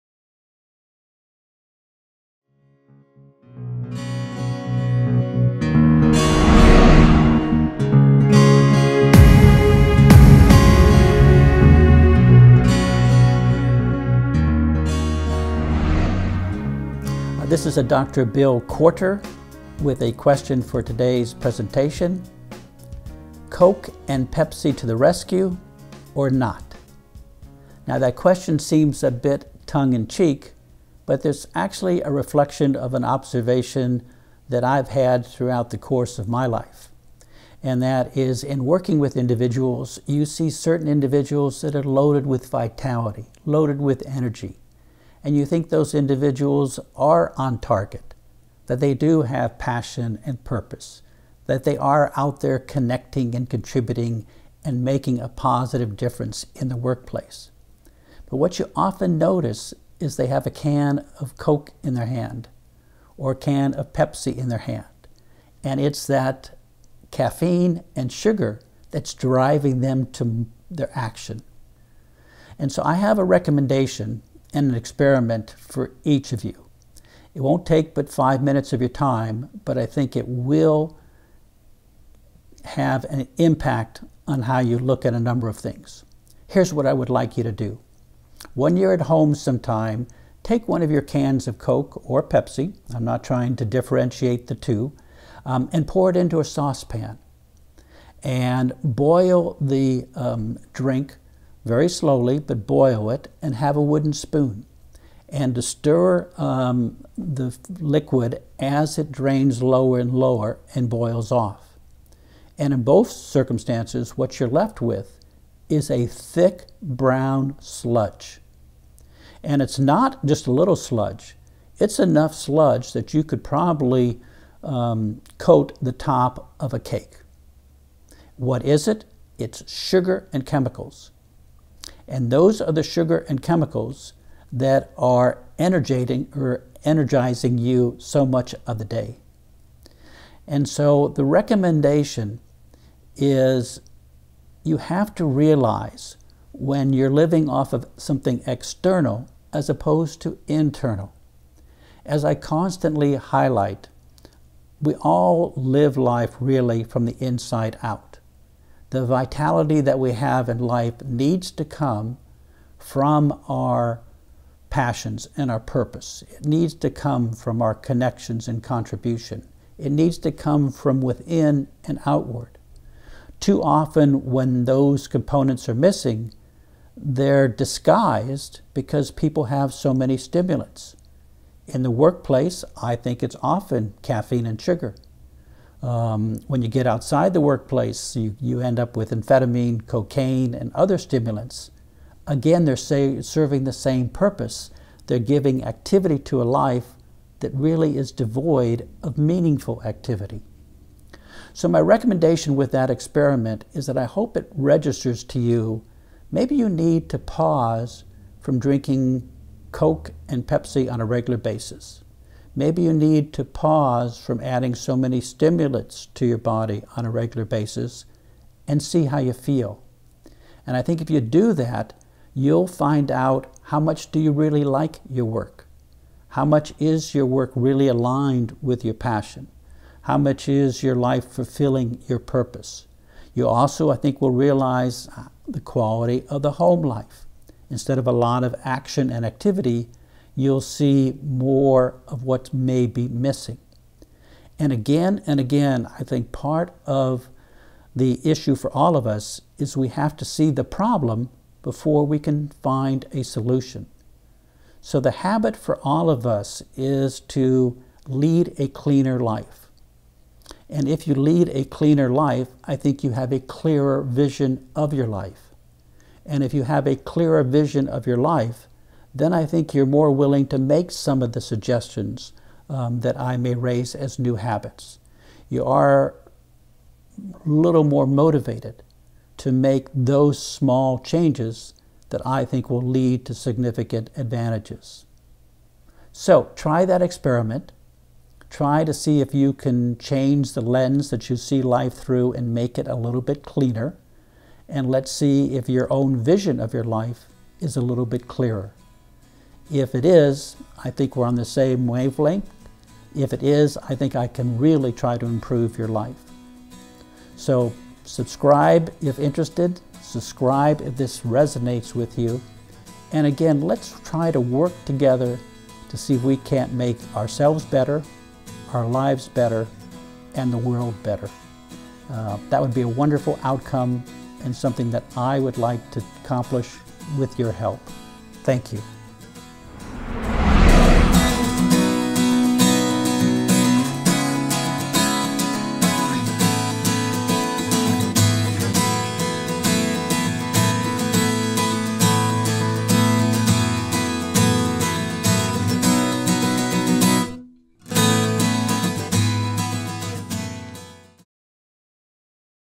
Coke and Pepsi to the rescue? Or not?
Do you depend on caffeine to get you through your day? Want to try an experiment for a cleaner life and a clearer vision?

Surviving A Toxic World - Video 25